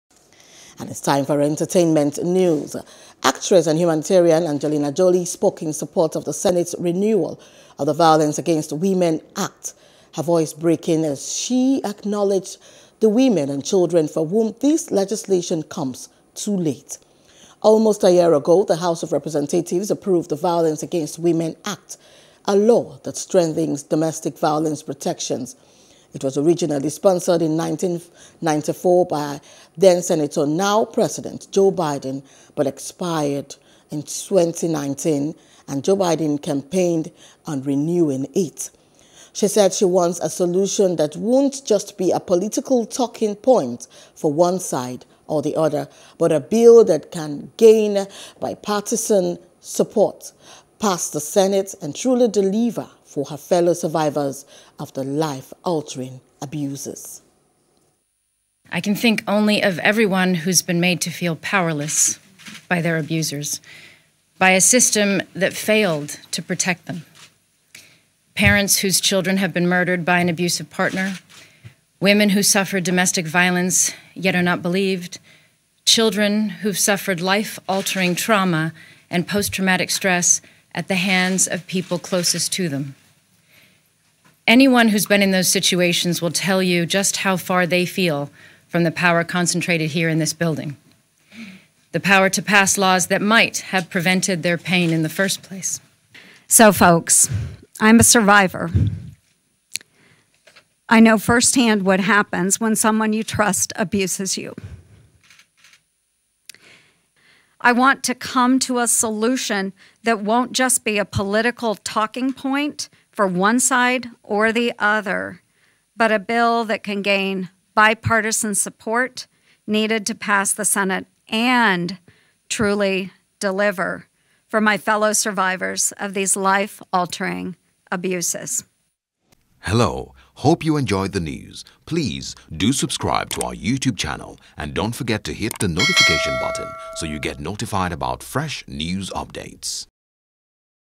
Domestic Violence: Emotional Angelina Jolie Advocates For Law | ENTERTAINMENT
Actress and humanitarian Angelina Jolie spoke in support of the Senate's renewal of the Violence Against Women Act, her voice breaking as she acknowledged the women and children "for whom this legislation comes too late."